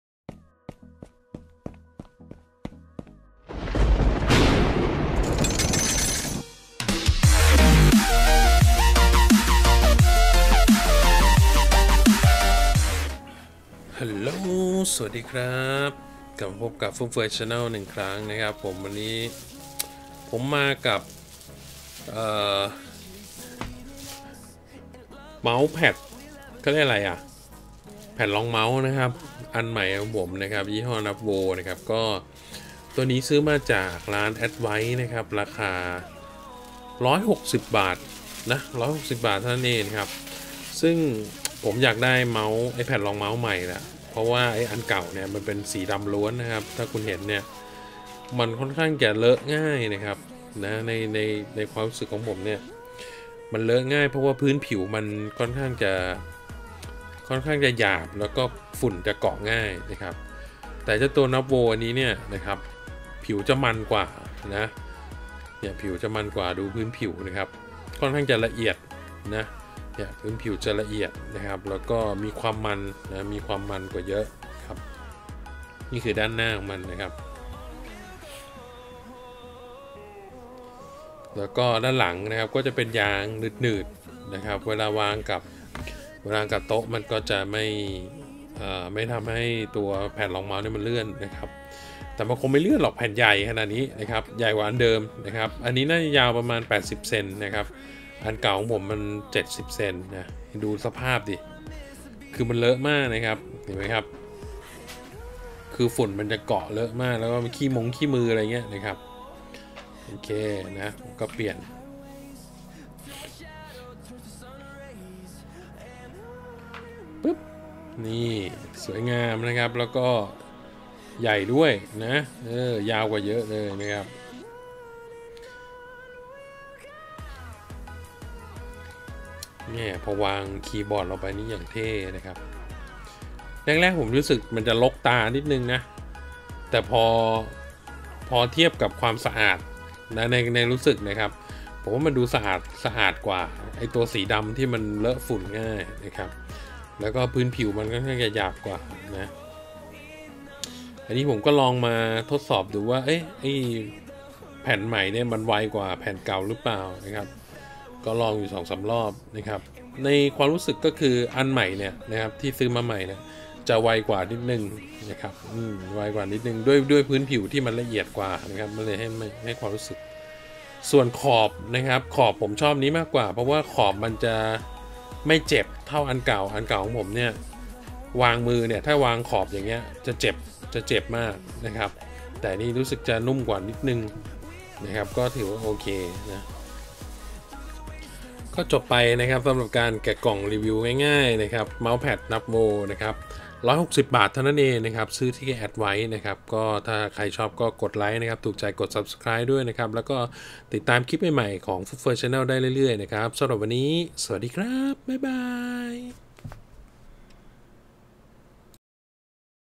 แกะกล่อง&รีวิว แผ่นรองเมาส์ NUBWO NP021 สีสันสวยงาม ยาวใหญ่!!!
ขนาดประมาณ 78 ซม. x 30 ซม. หนาประมาณ 3 มม.
ถือว่าเหมาะมากกับผมเลยหล่ะครับ เพราะผมชอบพื้นที่แผ่นรองเมาส์ที่มากๆ
เวลาใช้งานเมาส์จะได้ไม่ต้องมาพะวงว่าเมาส์จะหลุดจากแผ่นรองเมาส์หรือเปล่า
และที่สำคัญเจ้าแผ่นรองเมาส์รุ่น NP021 เป็นแบบสปีด คือเมาส์จะไวกว่าของเดิมที่ผมใช้อยู่ครับ
ปล. ราคา 160 บาทที่ผมได้มาไม่รู้ว่า advice เค้าลดราคาหรือยังไงนะครับ เพราะผมเจอเหลือแค่แผ่นเดียว แต่ผมเห็นที่อื่นราคาก็แพงกว่าพอสมควรเลย ยังไงลองหาดูกันนะครับผม